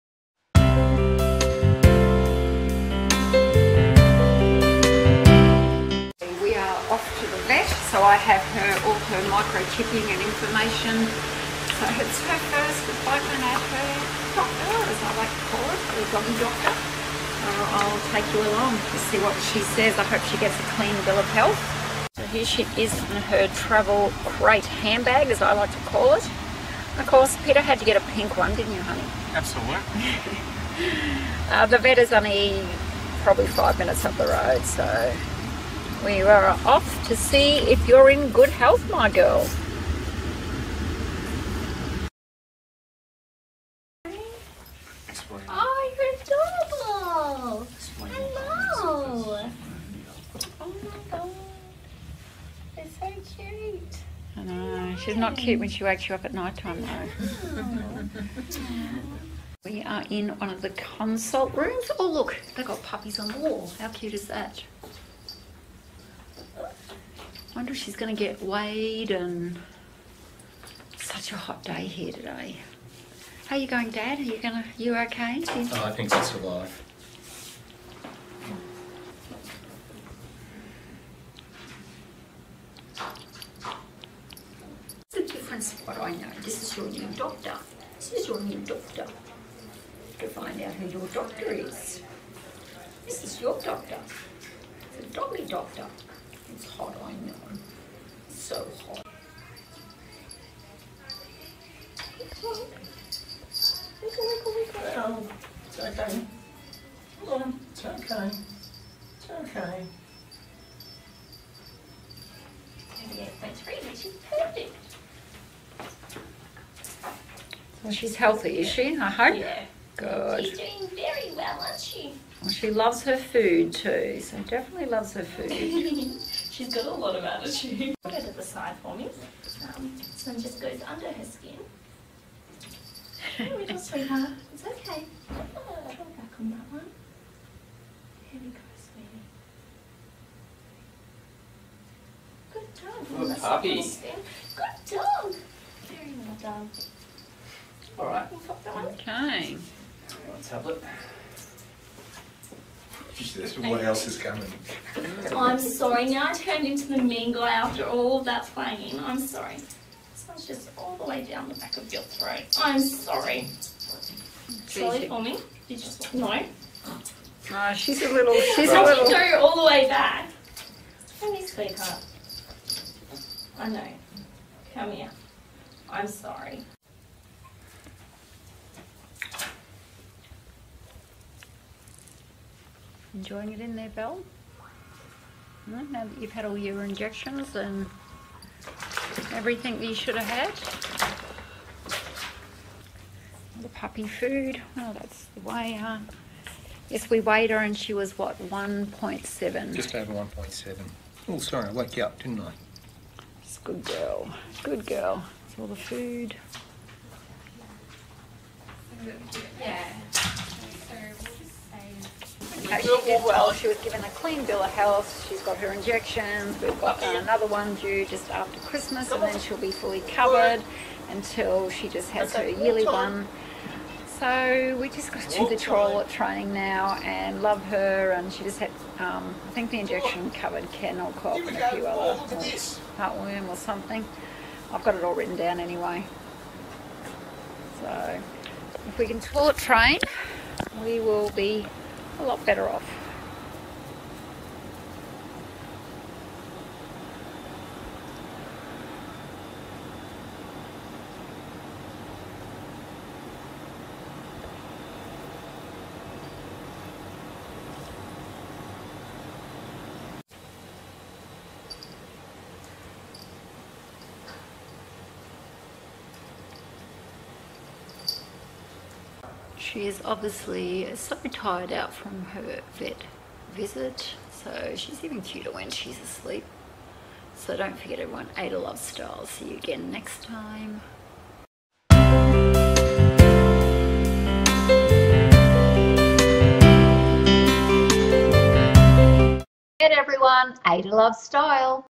Belle's first vet visit
In this video we take you along with Belle for her first visit to her vet. They were very helpful and treated Belle and ourselves very well.